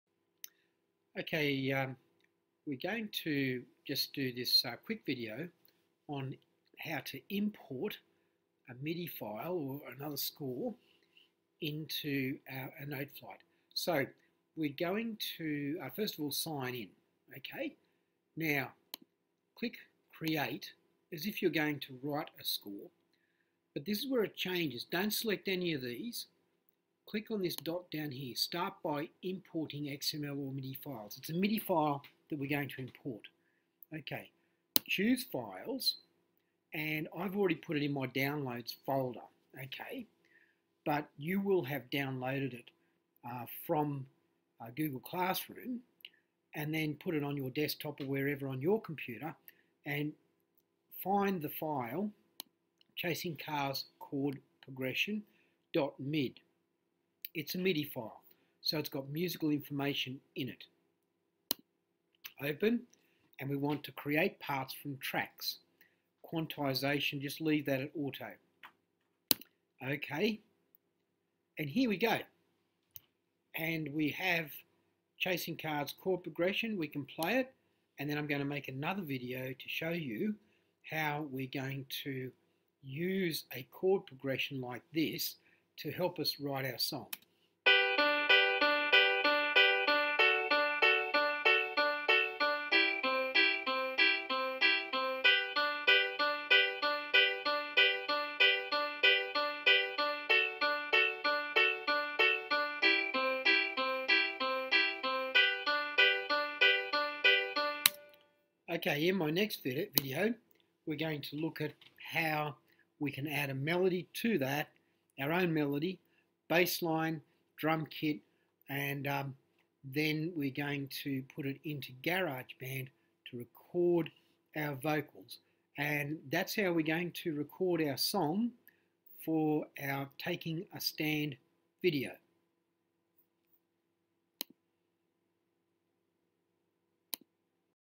How to import a midi file into Noteflight
It's a very handy thing to import a MIDI file into Noteflight